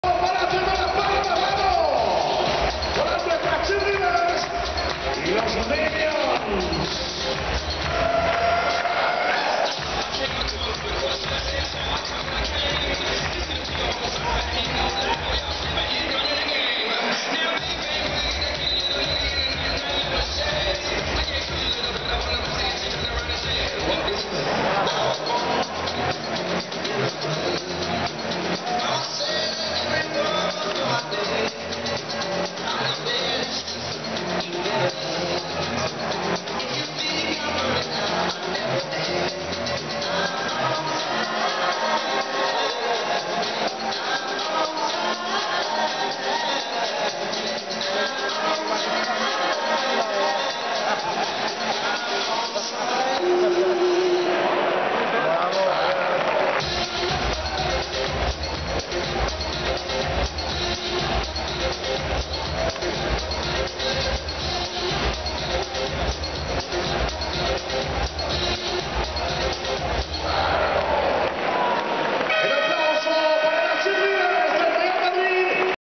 Los Minions & Las Real Madrid Cheerleaders
Actuacion de los personajes del cine con las Cheerleaders. Que gran momento